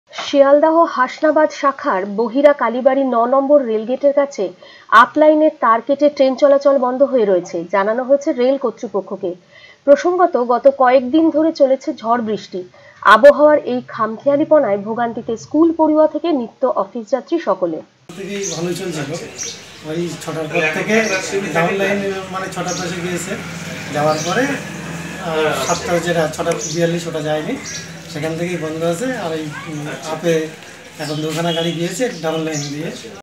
শিয়ালদহ হাসনাবাদ শাখায় ট্রেন চলাচল বন্ধ
শিয়ালদহ হাসনাবাদ শাখার বহিরা কালীবাড়ি 9 নম্বর রেল গেটের কাছে আপ লাইনের তার কেটে ট্রেন চলাচল বন্ধ